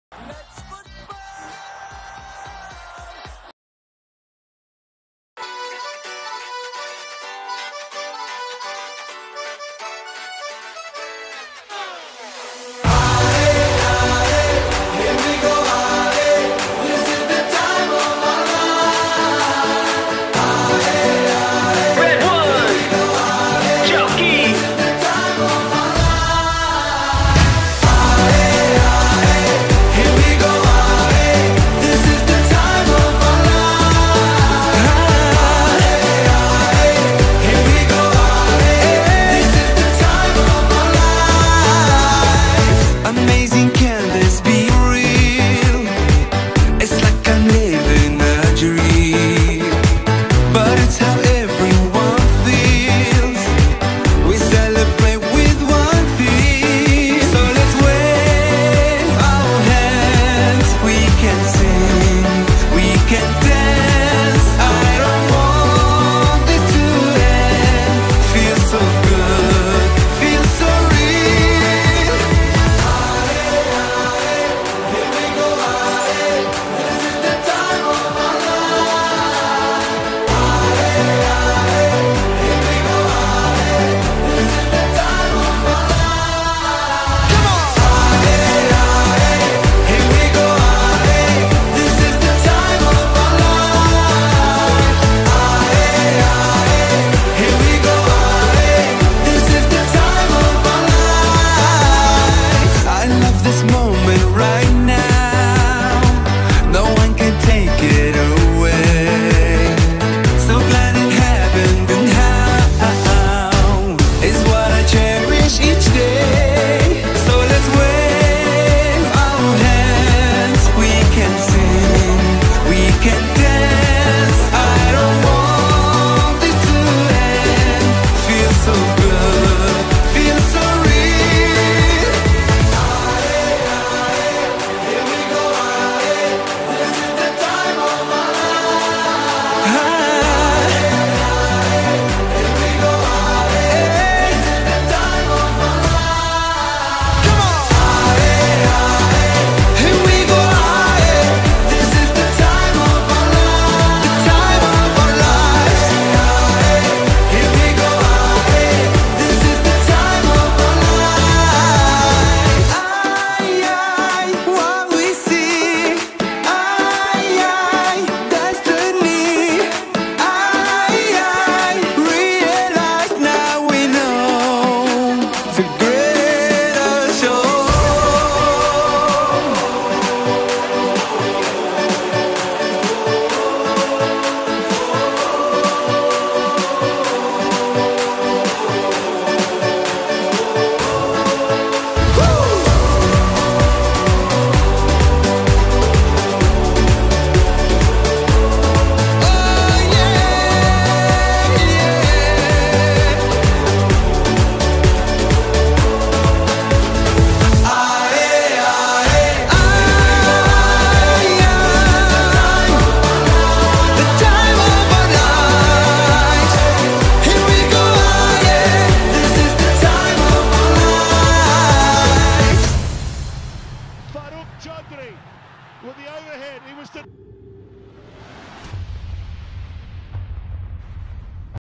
Jamshedpur fc🤍❤ || ISL theme song || fan made🥰😀
Isl jamshedpur fc promo song fan made for fun.